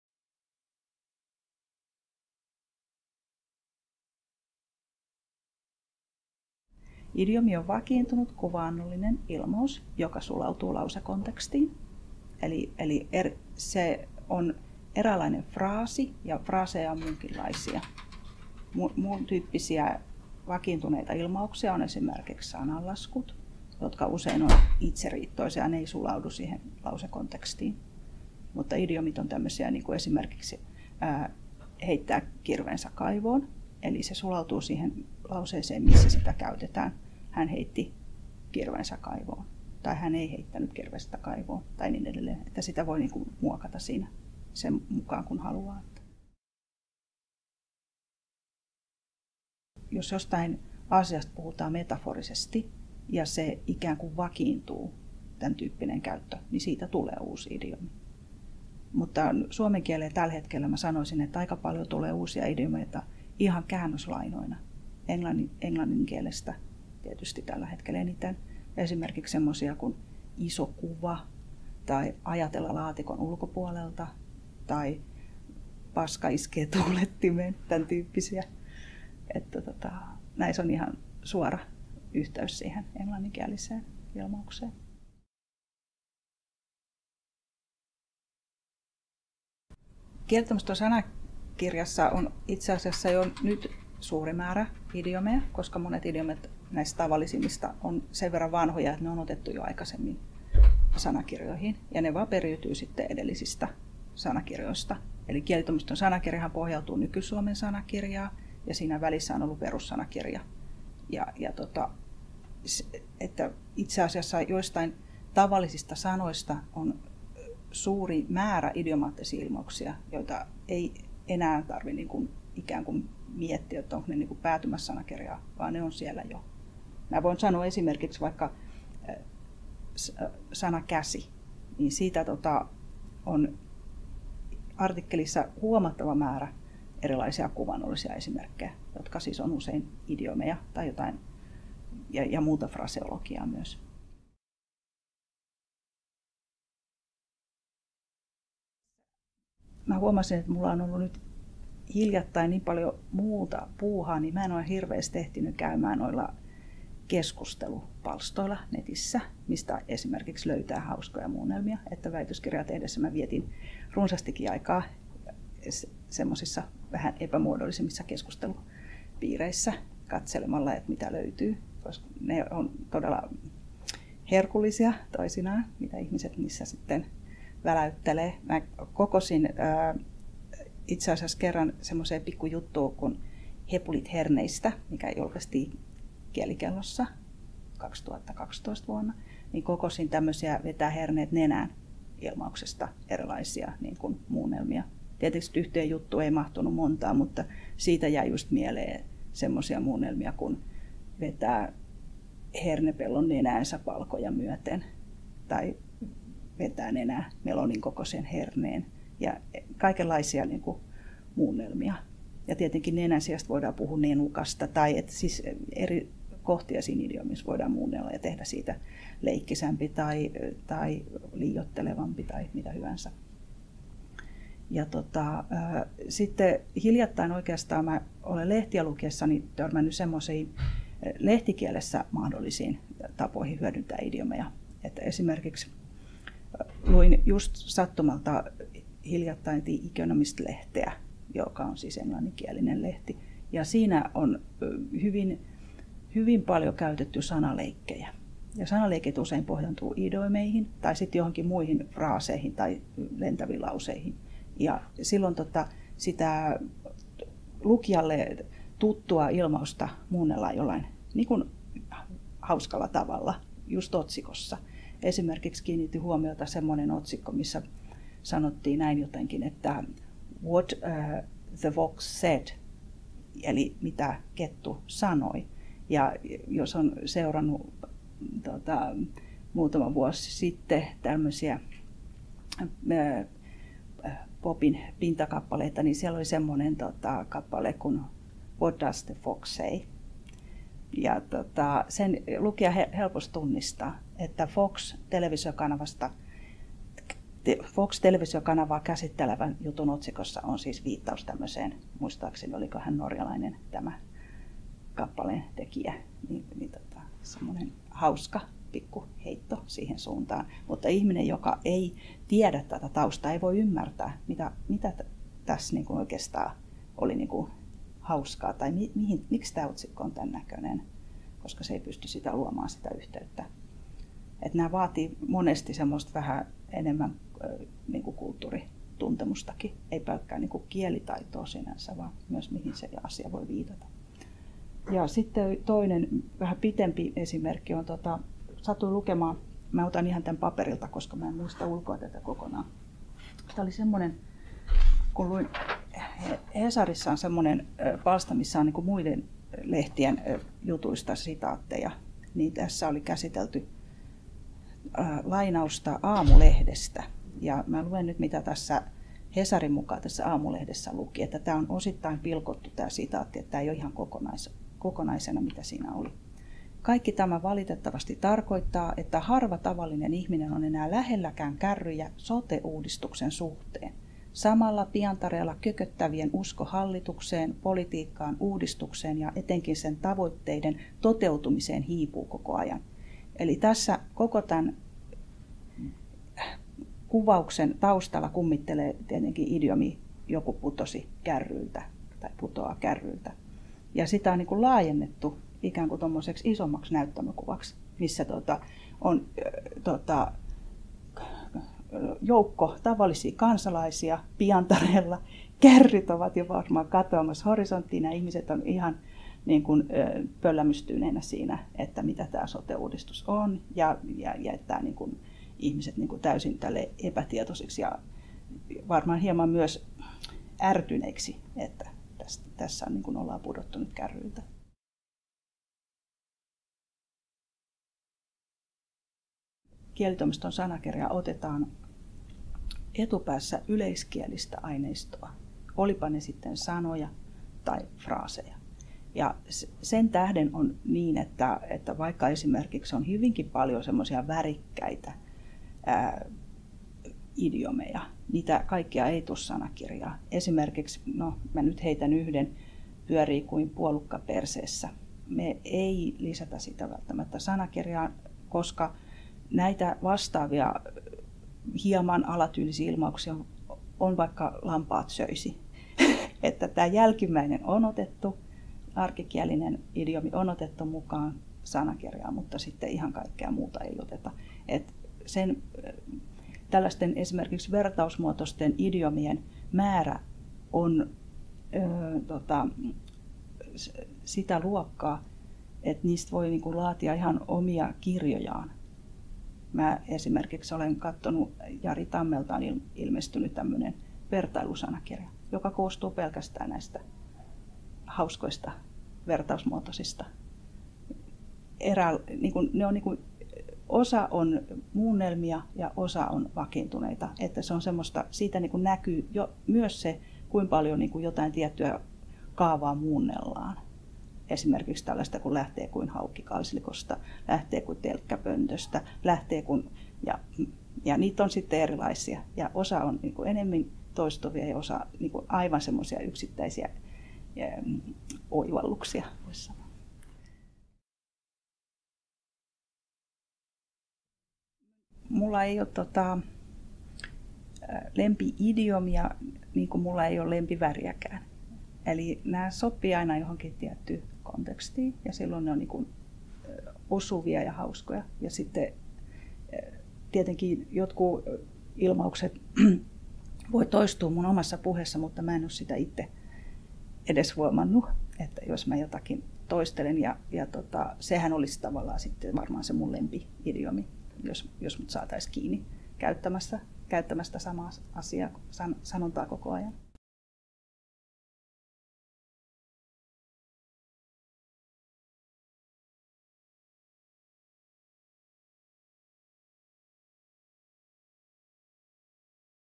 Tarja Riitta Heinosen haastattelu
Kielitoimiston sanakirjan päätoimittaja Tarja Riitta Heinonen kertoo idiomeista.